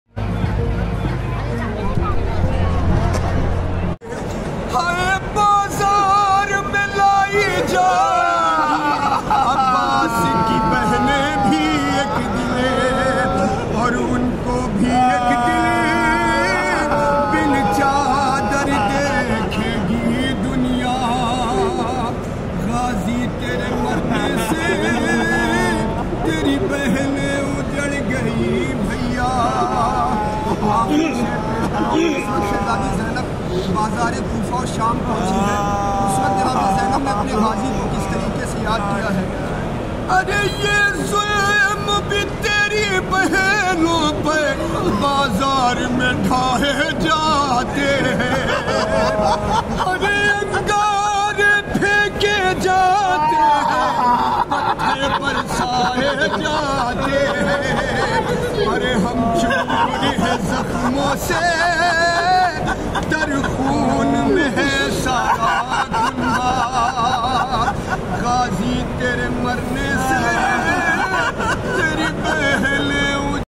Anjuman e Siphae Hussaini Ke Nohakhuwan | Zuhair Sultanpuri Sahab | Haram Hazrat Abbas
About This Video: Momneen Is Video Men Noha Pesh Kiya Gaya Hai Anjuman e Siphae Husaini Sultanpur Ke Nohakhuwan Janab Zuhair Sultanpuri Sahab

Ba Maqam Haram Hazrat Abbas As Karbala Iraq

Coverage Video Harmain Tv